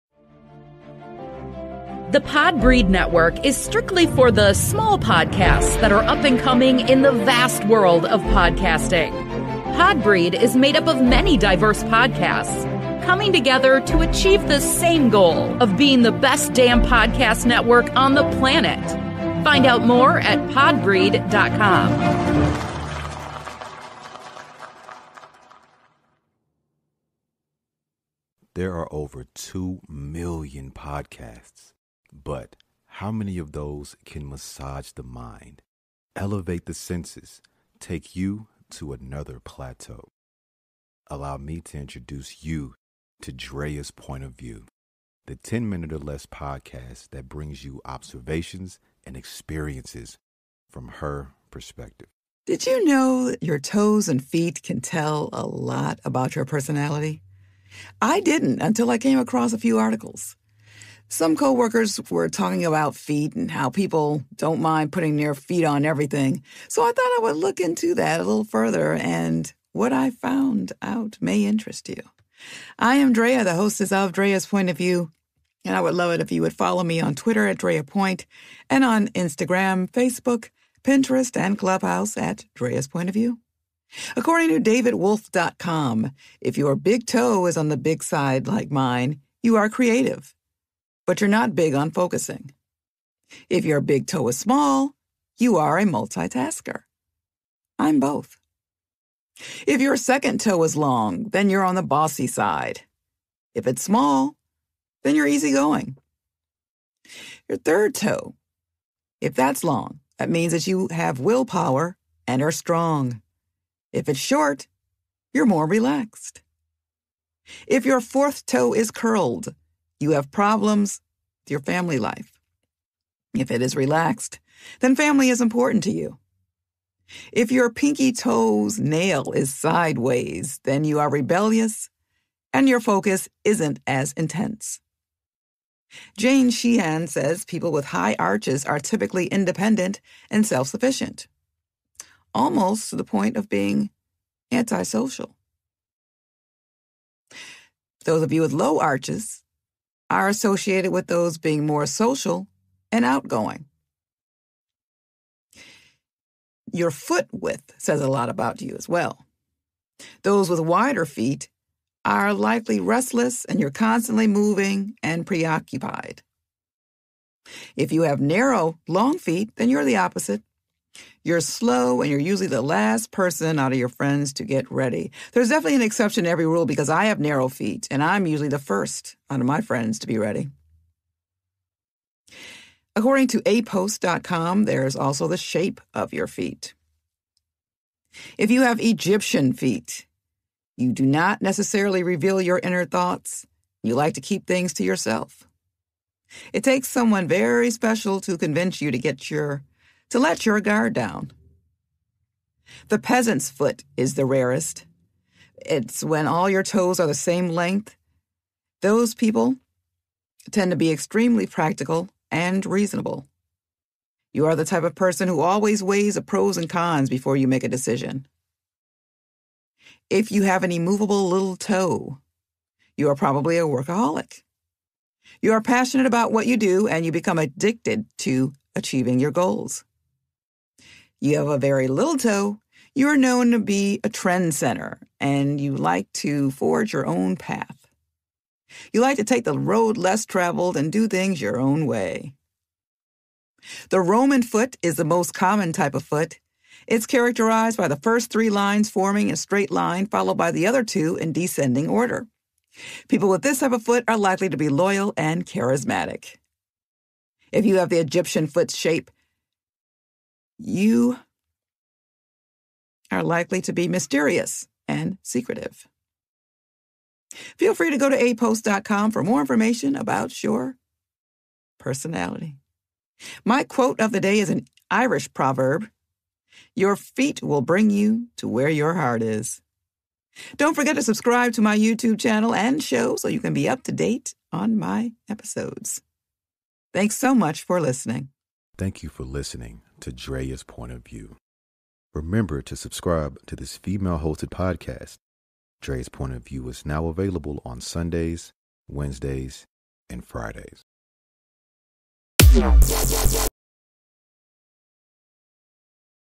Episode 375: What do your feet and toes say about your Personality?
This episode gives the characteristics of one who has certain toes and feet.